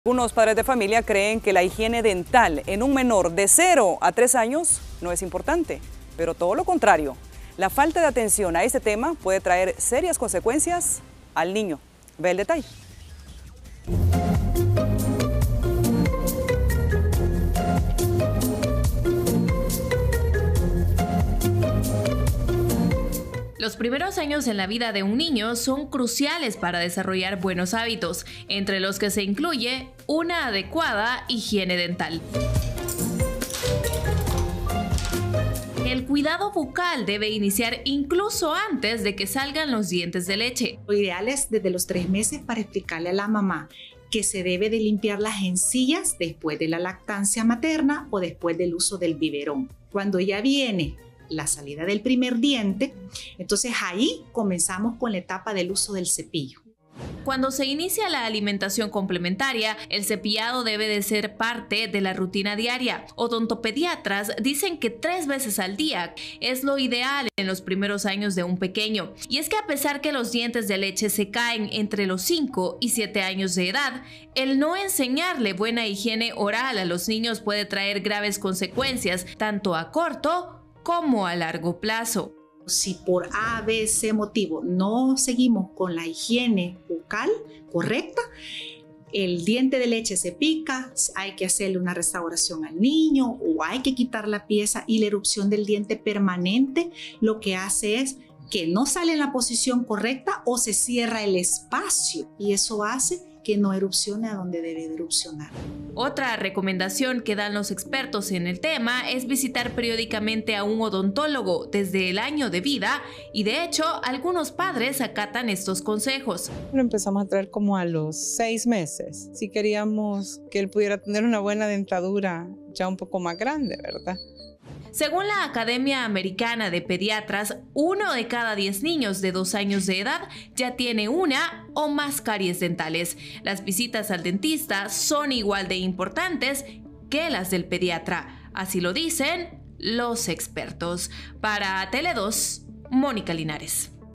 Higiene dental en niños de 0 a 3 años
Algunos padres de familia creen que la higiene dental en un menor de 0 a 3 años no es importante, pero todo lo contrario , la falta de atención a este tema puede traer serias consecuencias al niño. Veamos los detalles.